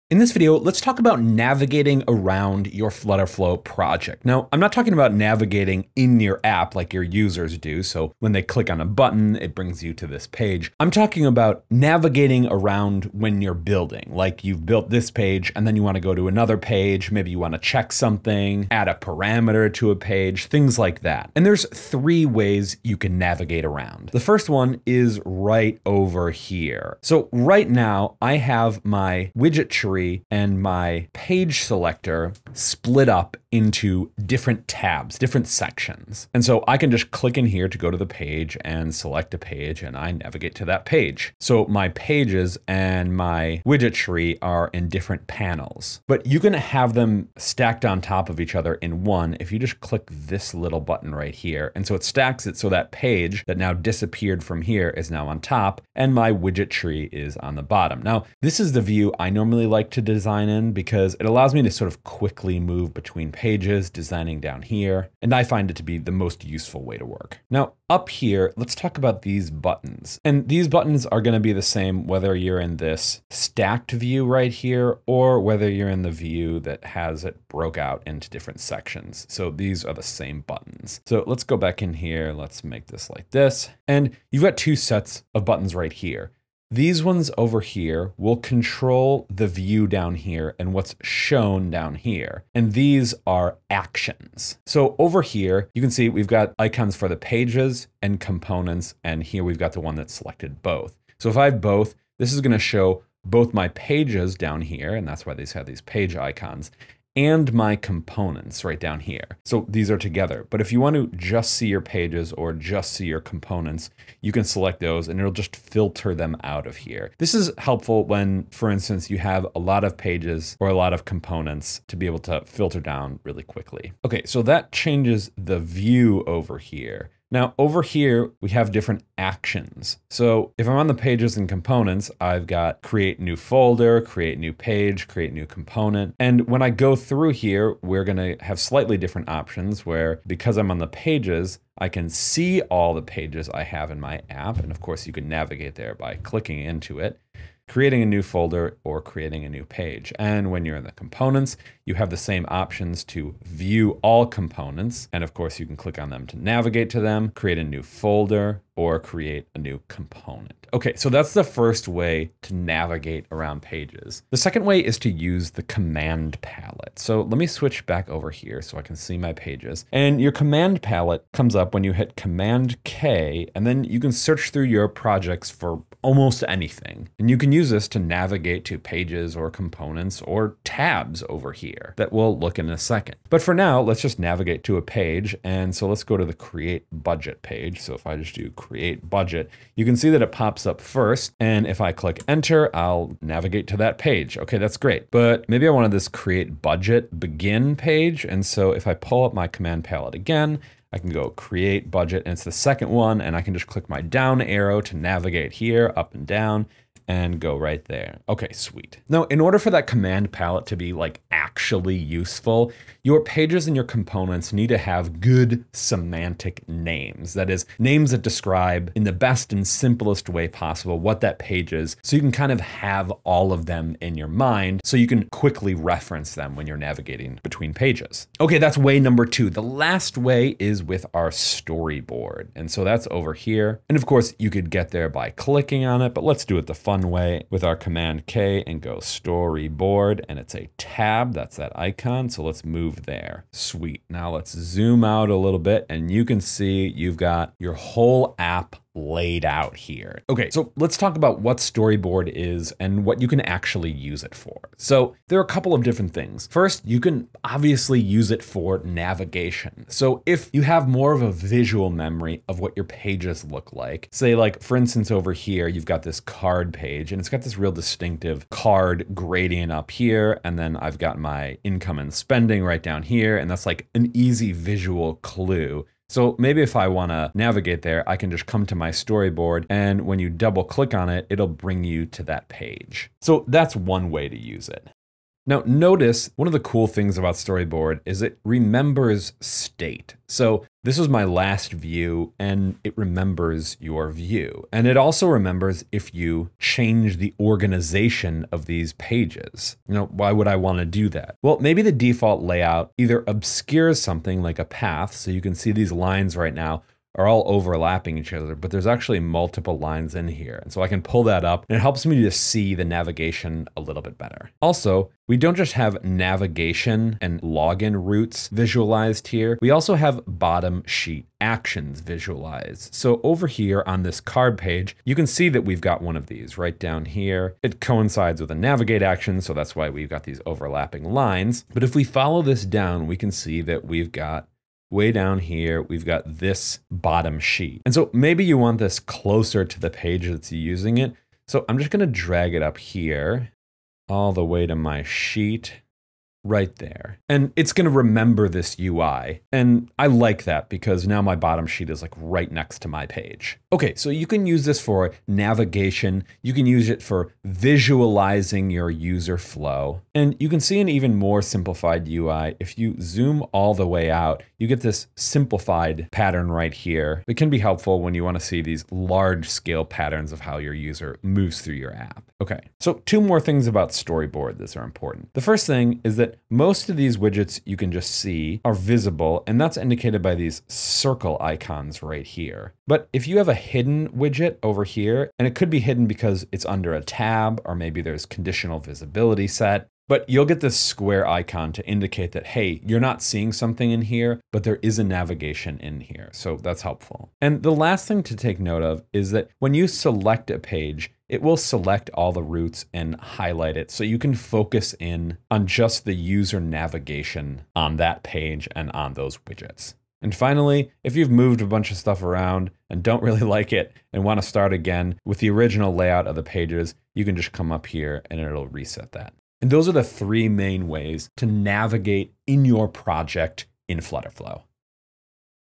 Navigating Pages & Storyboard | FlutterFlow University
In this FlutterFlow tutorial, the host introduces and explains the key concepts and methods for navigating around your FlutterFlow project when you're in the process of building. The video outlines three main ways to navigate: through the widget tree and page selector, the command palette, and using the storyboard feature. The host demonstrates each method in detail, elaborating on how to manage views, create new components, and navigate to specific pages. The video also emphasizes the importance of semantic names for pages and components for efficient navigation. The storyboard feature's function as a tool for visualizing user flow and its ability to remember states is highlighted, making this video a comprehensive guide to navigating your FlutterFlow project effectively.